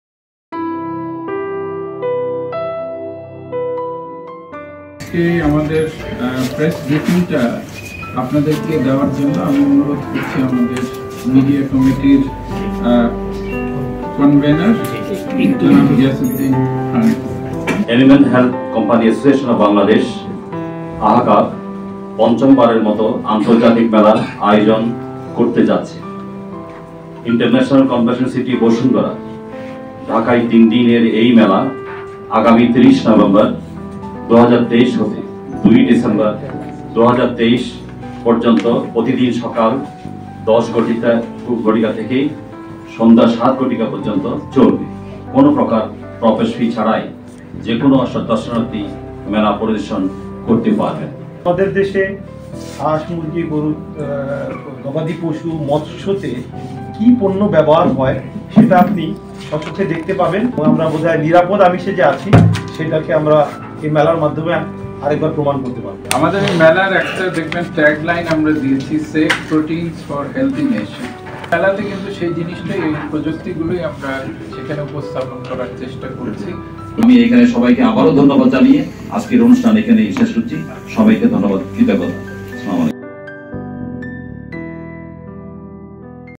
ahcab expo
A total of 215 companies from various countries, including Bangladesh, Egypt, Germany, India, Poland, South Korea, Romania, Thailand, Vietnam, and others, will participate to demonstrate their contributions to ensuring the supply of safe animal proteins to meet both local and international demands. Additionally, the event will address challenges related to disease and rearing of pet animals.